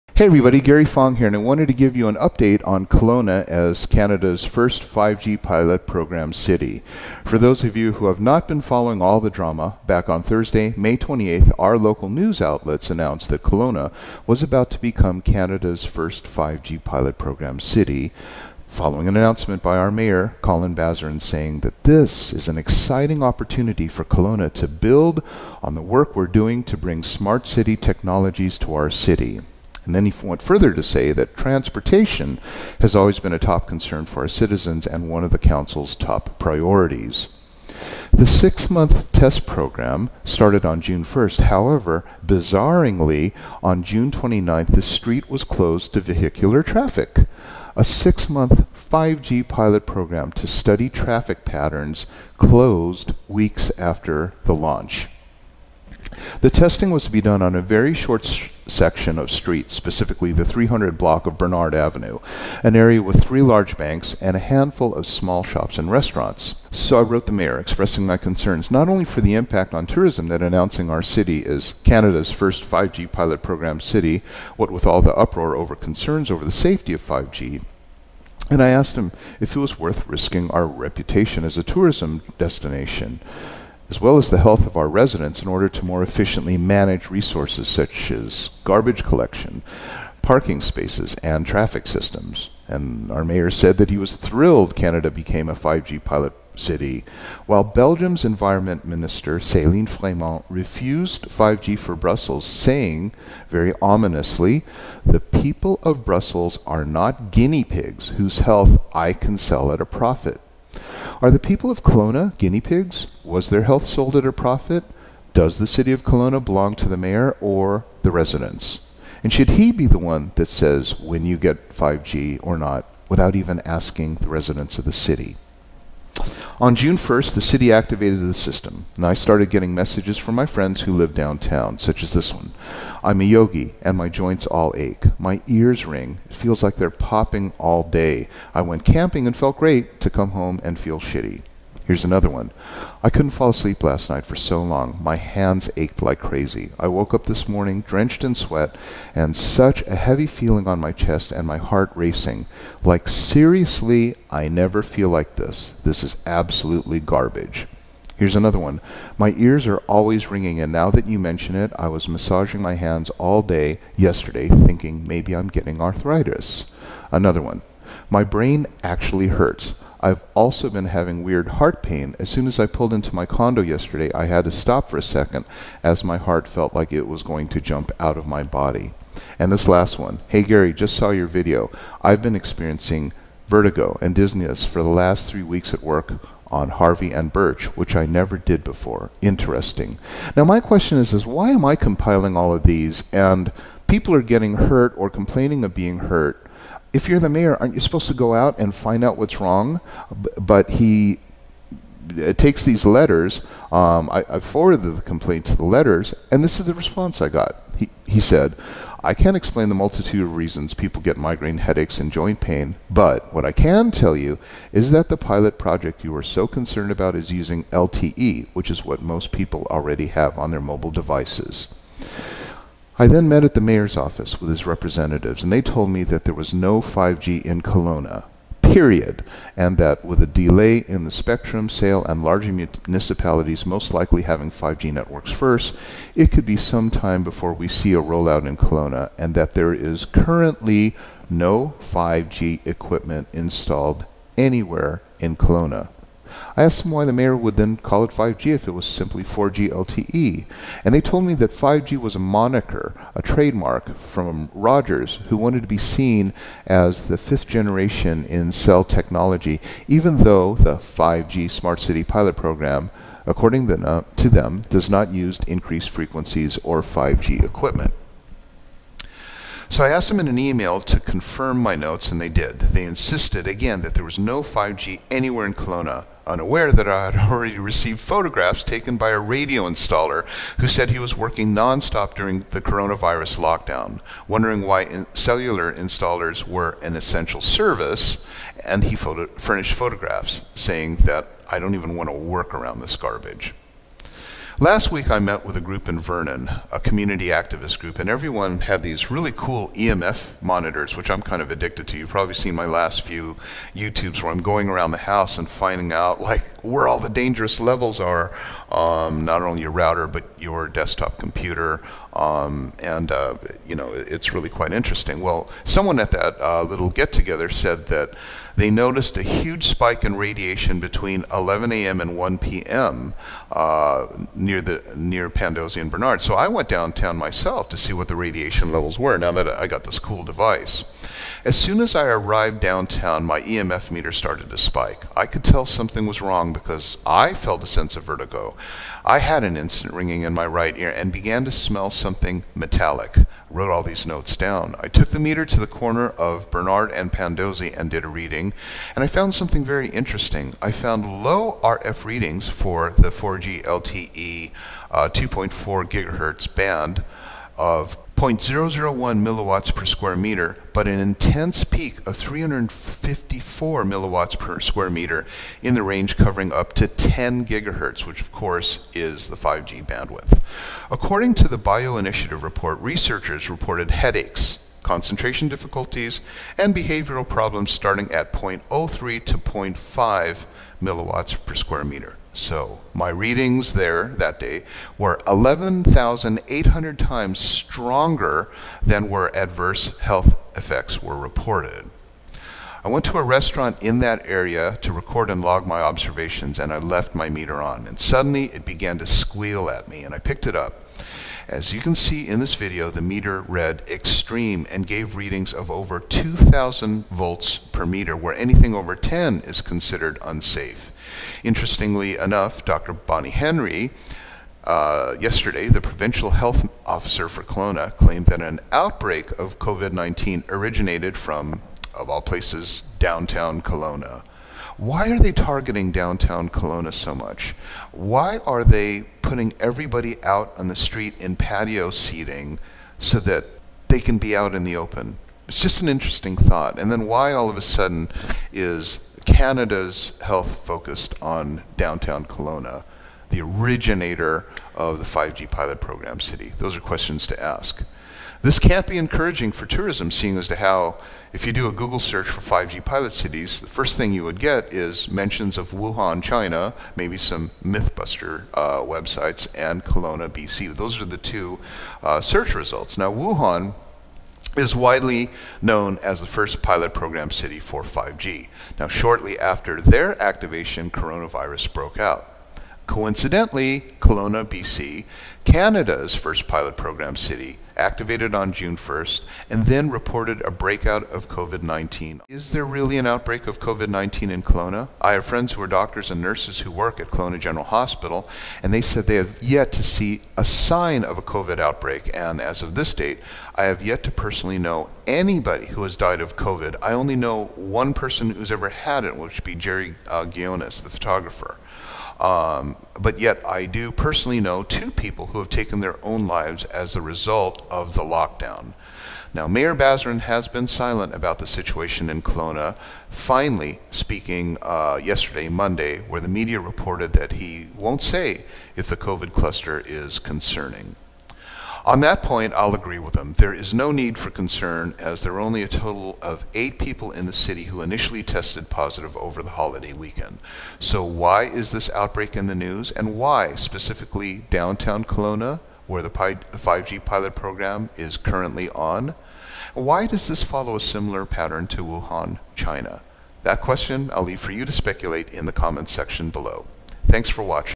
Kelowna Microwave Radiation Reading On Bernard Ave Downtown
High Microwave radiation levels in Kelowna caught on video.
On May 28, it was announced that Kelowna was to become "Canada's First 5G Program City to "monitor traffic patterns" with the "Lidar" traffic surveillance system. On June 29th, just weeks after implementing this traffic monitoring system - the area disallowed vehicles.
On June 1st, when the system was implemented, many people were reporting symptoms, most commonly "intense migraines" and "joint pain".
I went to the area with my EMF meter. Watch how high the levels went.